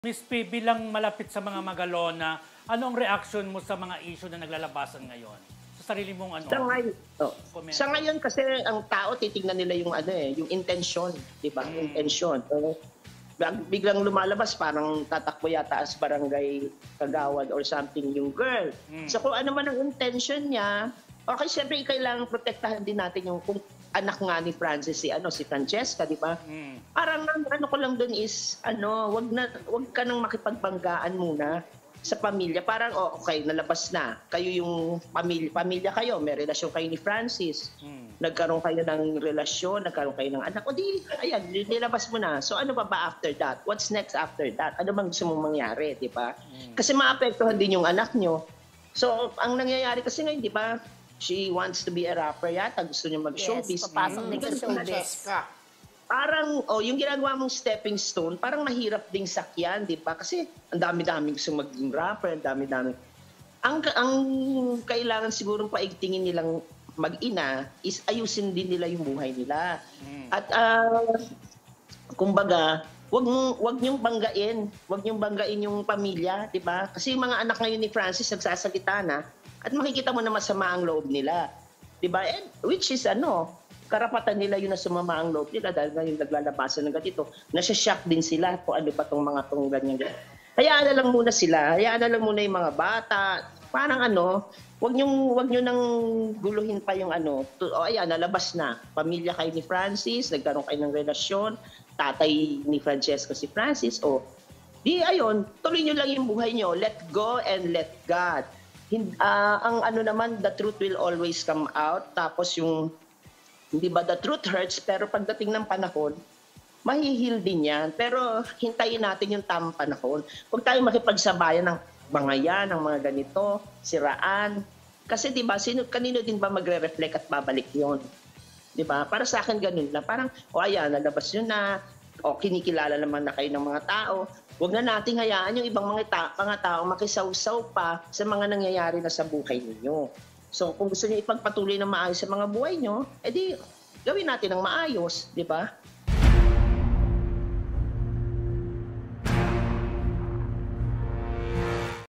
ABEGAIL RAIT PINAYUHAN ‘WAG NA MAKIPAGBANGGAN, AYUSIN BUHAY | MARISOL ACADEMY QUICKIE
‘ANONG INTENSYON?’

Pinayuhan ni Pilar Mateo na dating publicist ni Francis Magalona, si Abegail Rait na huwag nang makipagbanggan at ayusin na lang ang kanilang buhay mag-ina upang hindi rin maapektuhan ng mga basher ang anak umano nila ng Master Rapper na si Gaile Francesca.